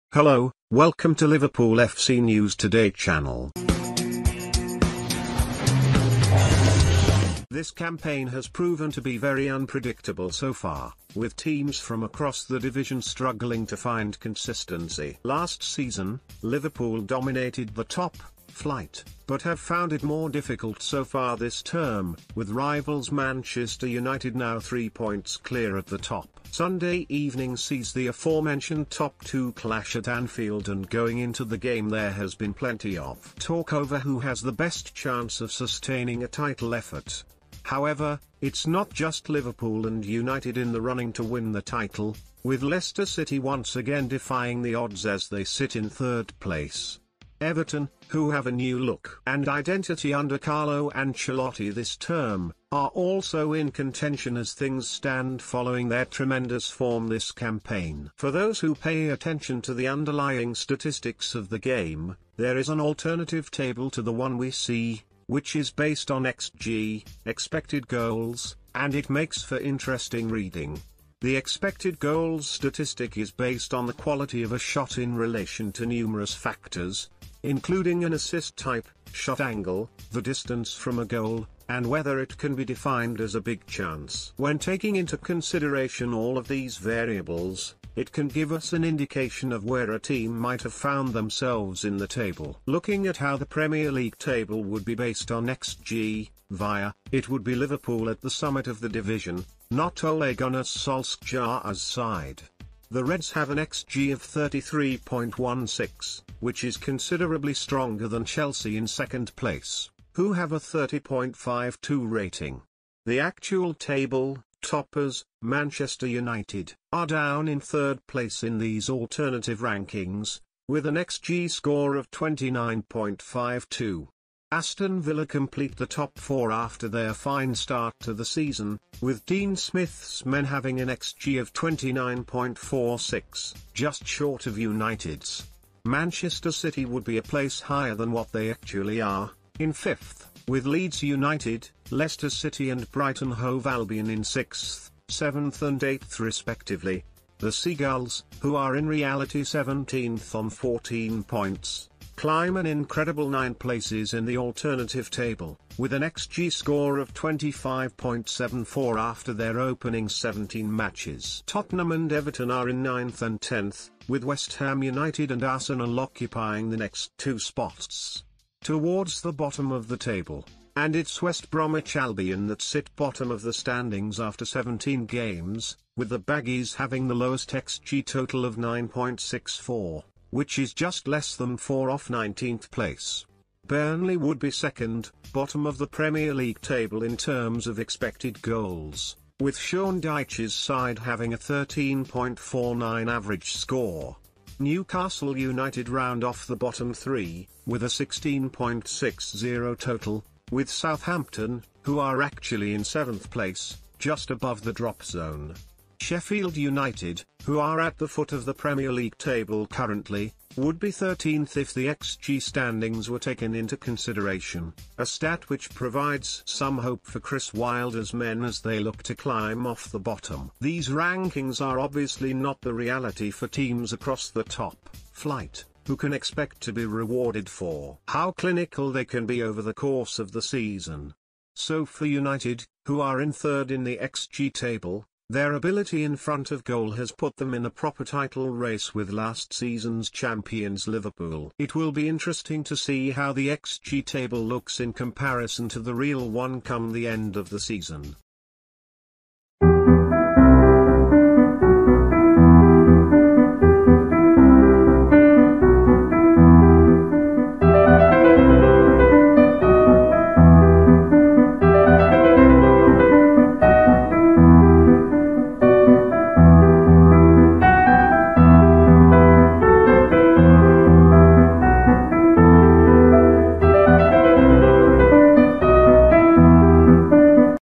Liverpool ahead of Man Utd in alternative stats-based Premier League table - news today
This campaign has proven to be very unpredictable so far, with teams from across the division struggling to find consistency. Last season, Liverpool dominated the top-flight, but have found it more difficult so far this term, with rivals Manchester United now three points clear at the top. Sunda...